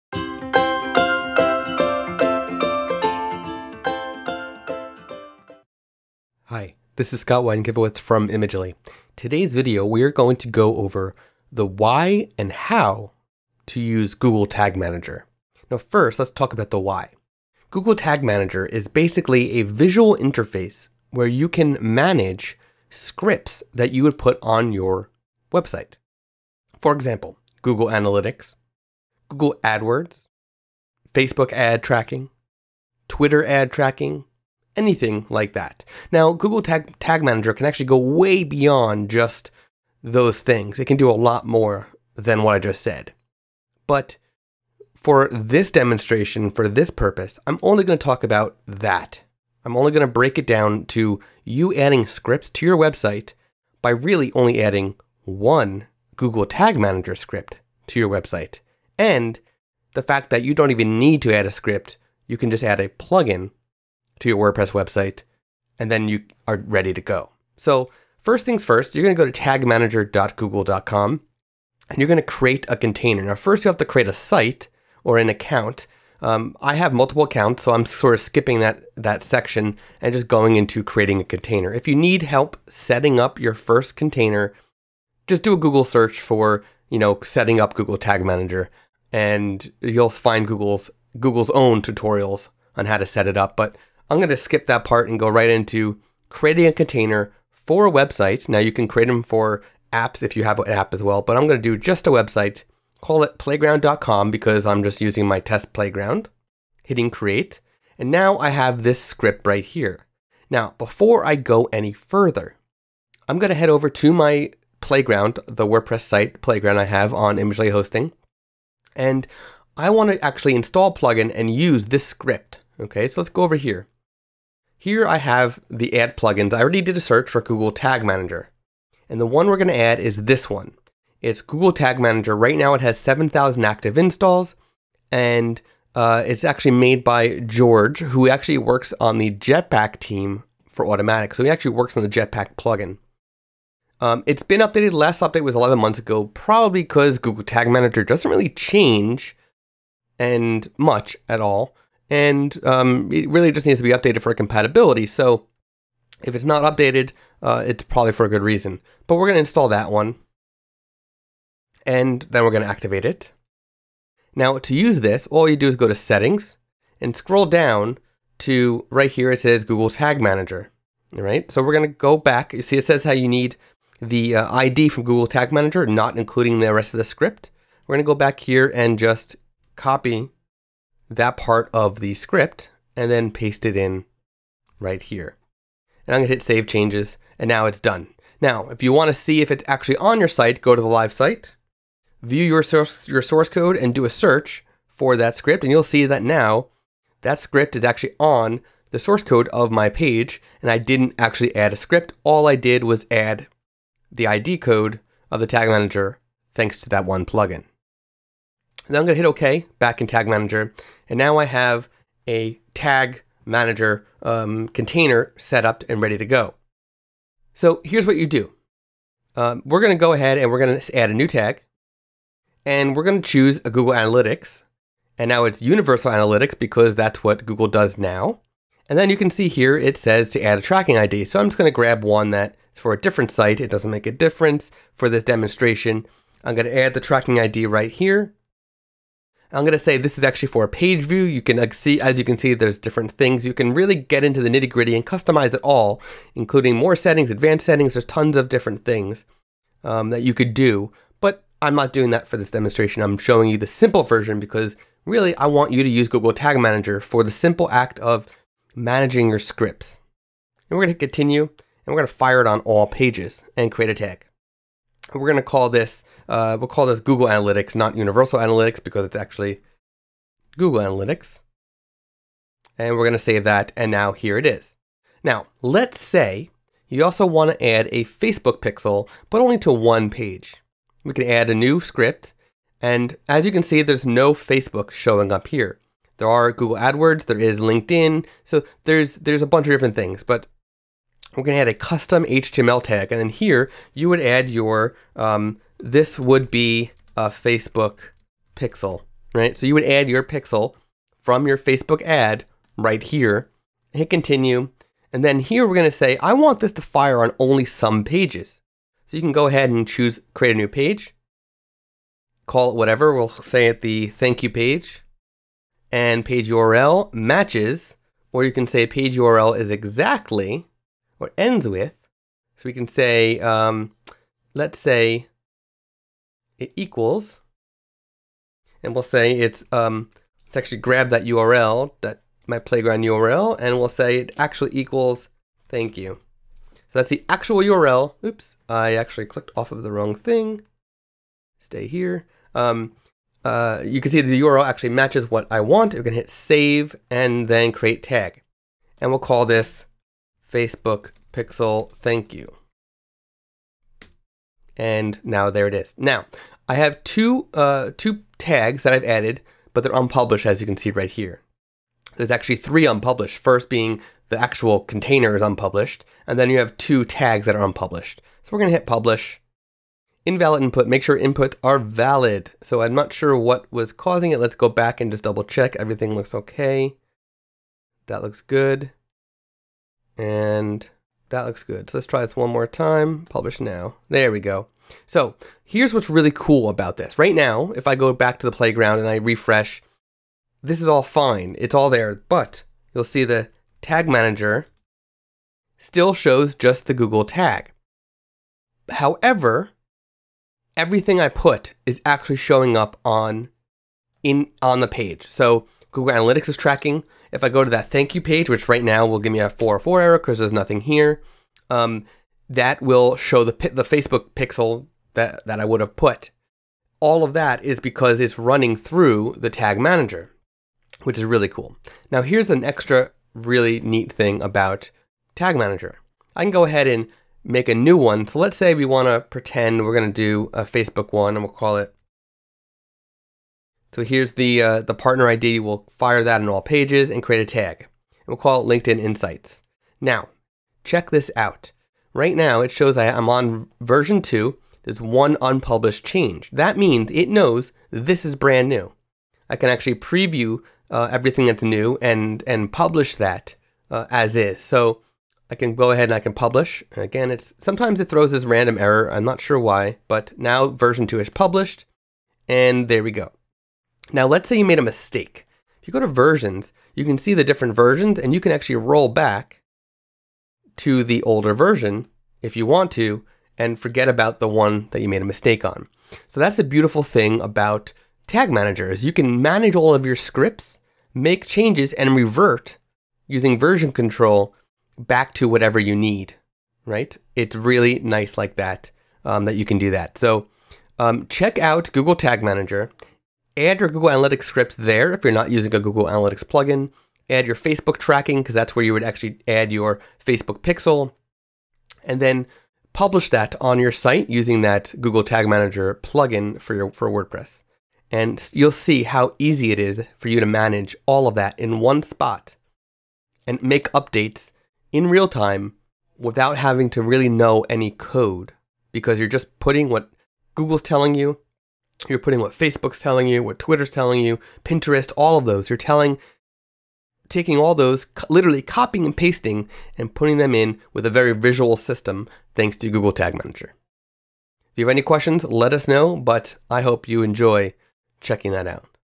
Why To Use Google Tag Manager & How To Set It Up
Google Tag Manager is a huge benefit for many photographers.

We say that because it simplifies the task of adding scripts to your website.

It also does so in a very visual way, which photographers can easily understand.